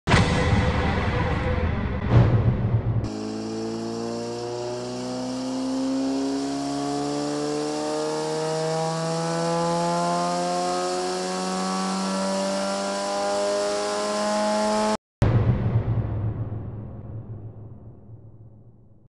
HMF: Suzuki GSX-R 1000
During the design phase of creating the '07-'08 Suzuki GSX-R 1000 we found that the left side muffler wasn't doing much but keeping the safety police happy. We designed a single, right side exhaust that drops some serious weight from the bike & delivers 14hp. Even better, the HMF Street Series still uses the stock O2 sensor, so a fuel controller is not required.